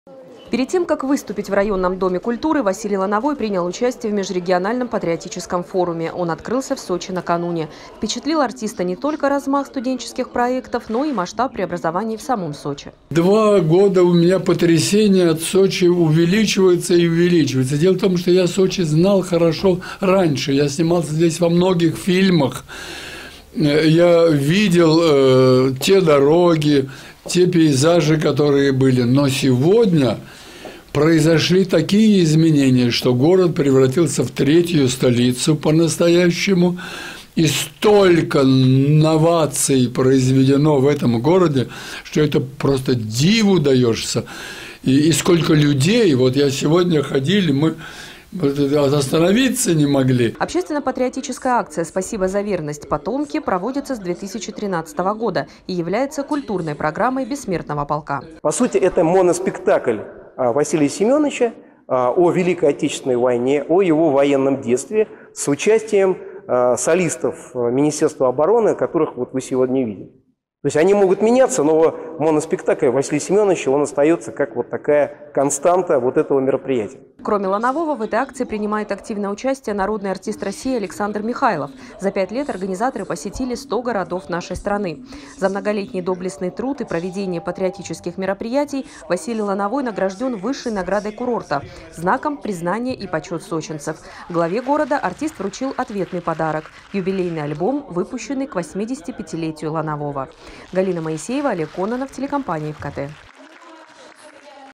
Народный артист СССР получил высшую городскую награду в Сочи. Новости Сочи Эфкате
Актер и общественный деятель Василий Лаановой прибыл на курорт в рамках общественно- патриотической акции «Спасибо за верность, потомки». За многолетний доблестный труд и проведение патриотических мероприятий Василий Лановой награжден высшей наградой города - знаком «Признание и почет сочинцев». Главе курорта артист вручил ответный подарок - юбилейный альбом, выпущенный к 85-летию Ланового.